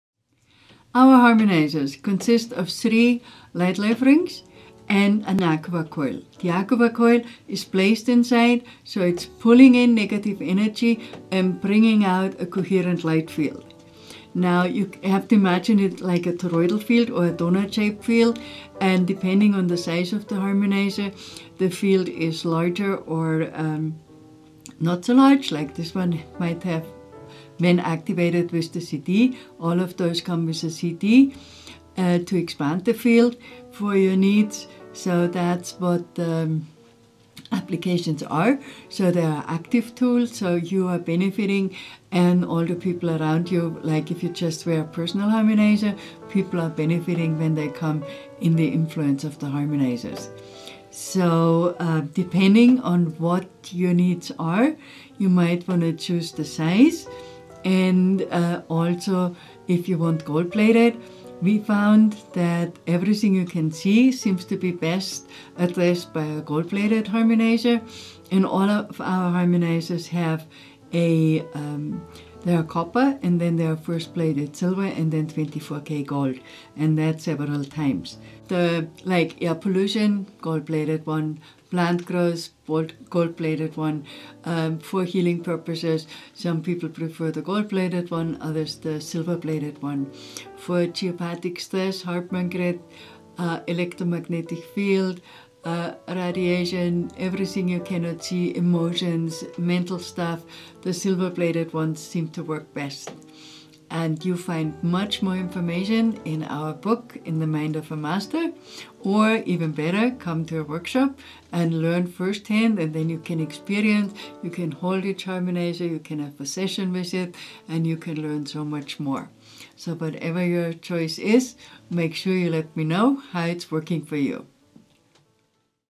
Harmonizers and the difference between gold and silver plating.
Katharina Spurling-Kaffl explains the Light-Life® Technology Harmonizers©